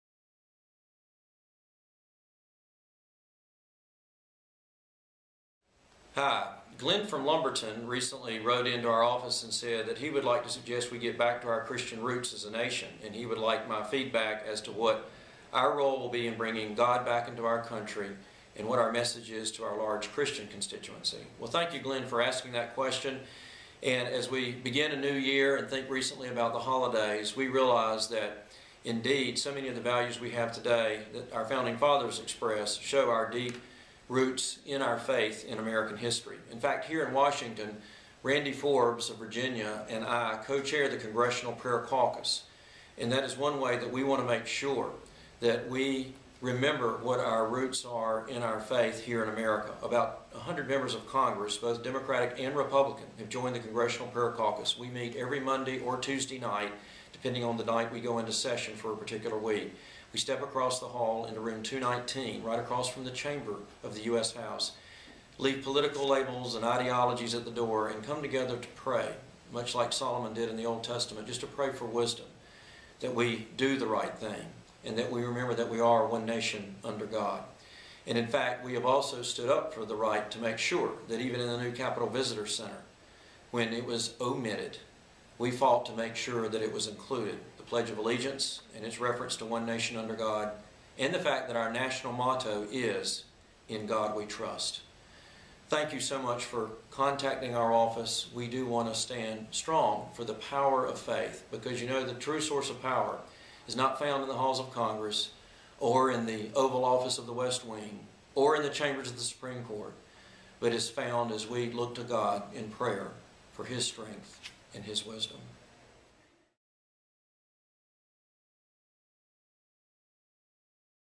Across the Desk from Rep. Mike McIntyre -- God and Country
Glenn from Lumberton aske "What role will you play to bring God back into our Country?"